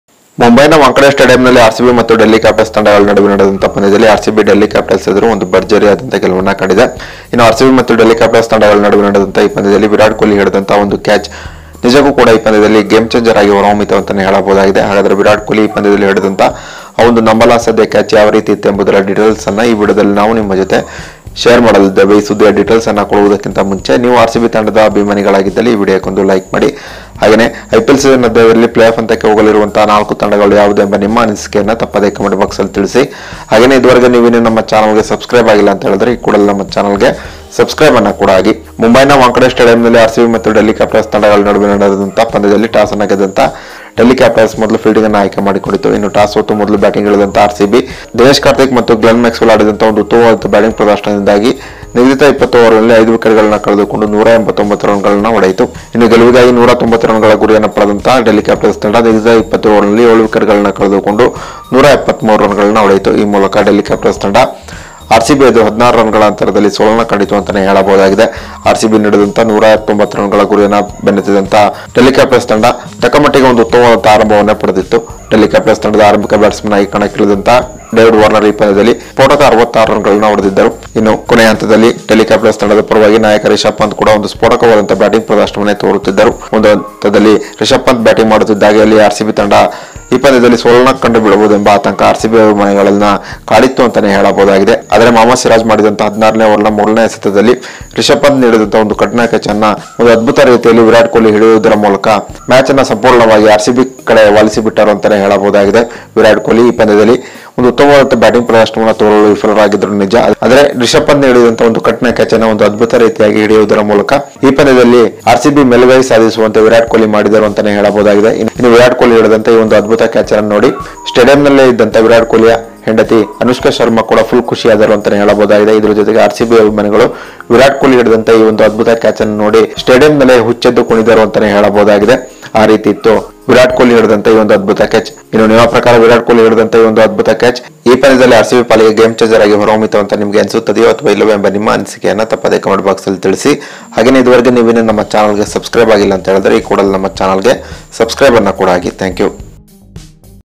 ಮ್ಯಾಚ್ ಗೆಲ್ಲಿಸಿದ ಕ್ಯಾಚ್.!! ಕೊಹ್ಲಿ ಹಿಡಿದ ಕ್ಯಾಚ್ ನೋಡಿ ಡೆಲ್ಲಿ ಟೀಂ ಕಂಗಾಲು | RCB | Virat Kohli
Images Courtesy by Star Sports and BCCI / IPL